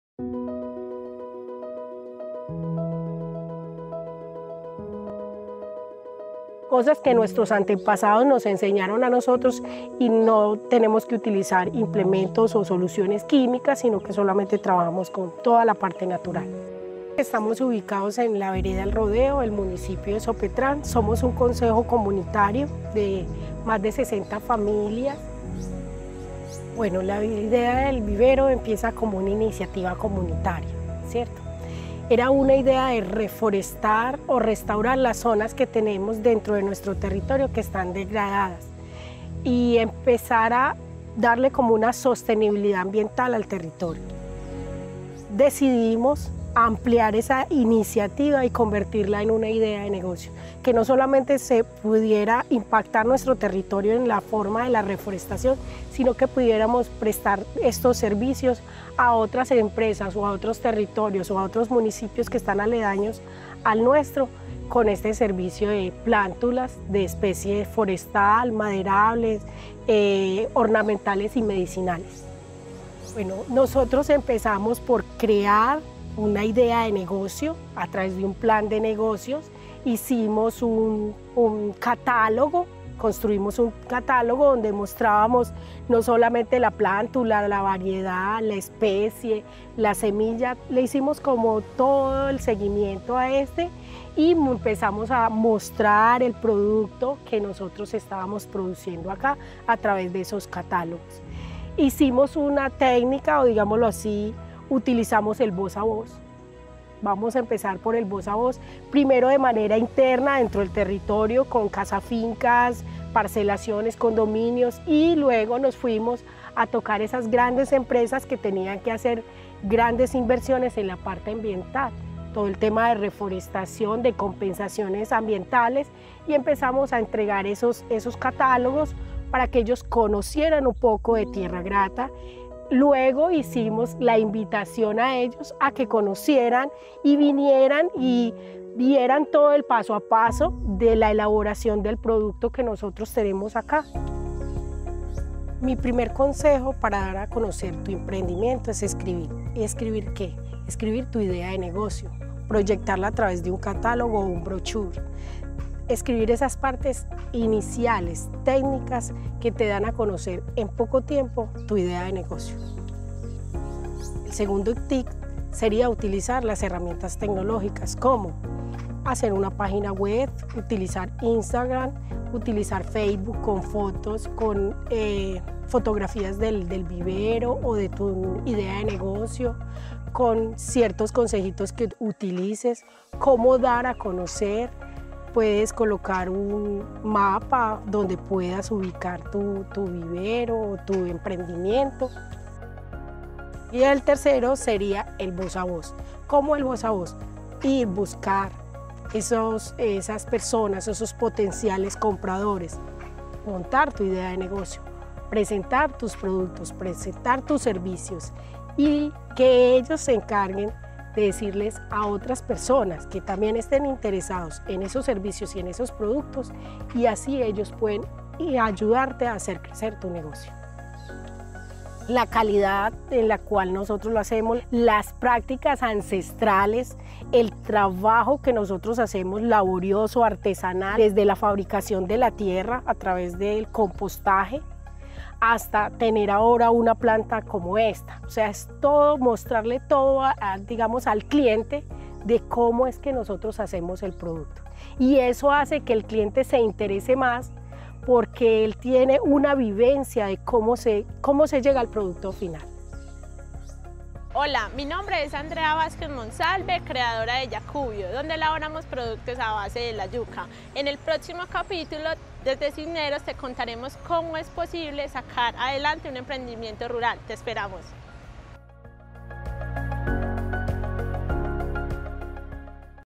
Capítulo 4 - Tierra Grata, un emprendimiento lleno de amor por la naturaleza.
Sabiduría ancestral y conocimiento que ha pasado de generación en generación con el fin de darle sostenibilidad ambiental al territorio.

@Tierragrata es un emprendimiento que impacta la vida de las comunidades aledañas y que empezó como una idea de transformación y de amor por la naturaleza.

No te pierdas las recomendaciones del final.⬆️